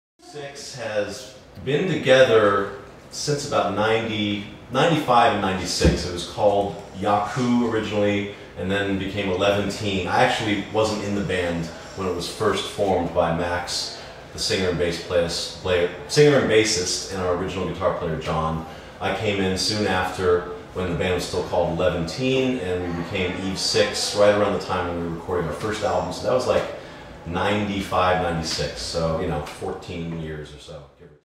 EVE6 INTERVIEW: How long has Eve 6 been together?
Interviews Tony Fagenson, drummer of EVE6.  He answers the question: How long has EVE6 been together?  Interview by Barbara Thompson, Video by Jospeh Grabiec